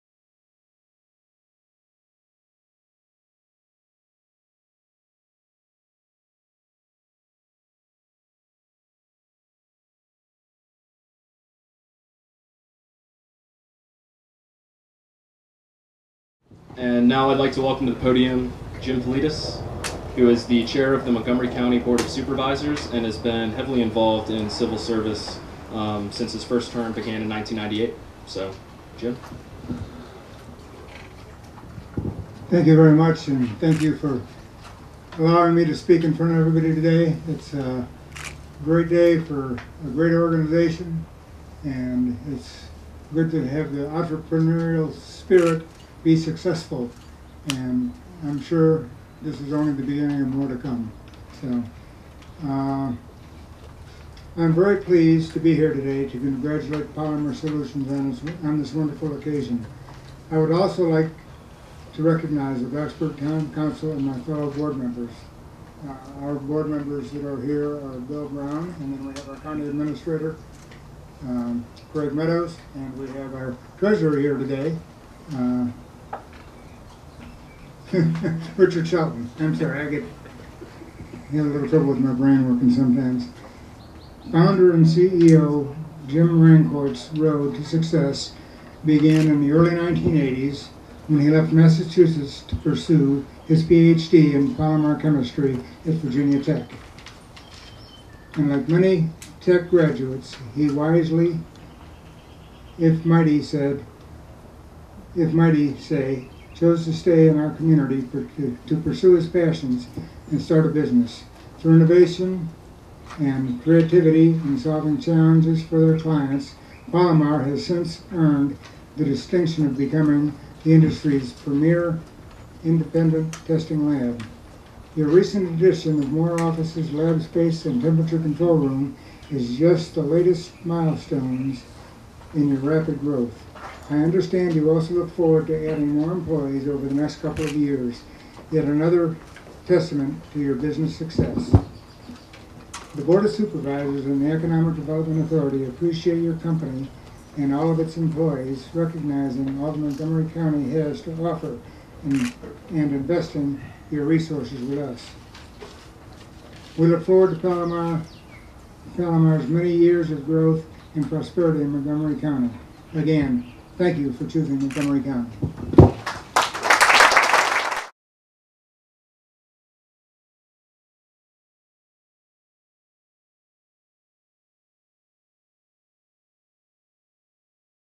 Jim Politis, Chair Montgomery County - Polymer Solutions Incorporated (PSI) Ribbon Cutting Ceremony
Ribbon cutting ceremony held on July 19, 2011 to celebrate Polymer Solutions Incorporated's (PSI) recent expansion in the Blacksburg Industrial Park.  Speaker Jim Politis, Chair of the Montgomery Board of Supervisors, provides remarks.